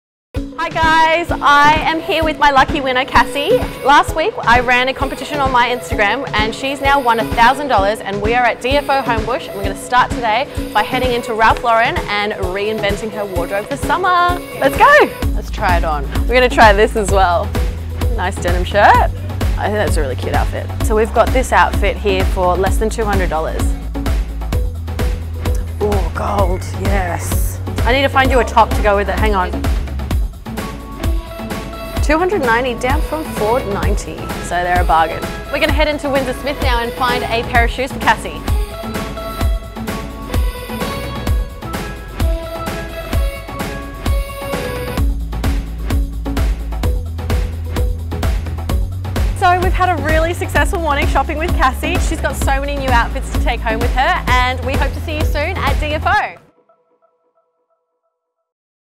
Helen Chik & Cassie at DFO Homebush
Join fashion influencer Helen Chik as she takes DFO newbie, Cassie around our Homebush centre.

Think you can out shop these two? Sign up to be a 'DFO Insider' for exclusive information and offers.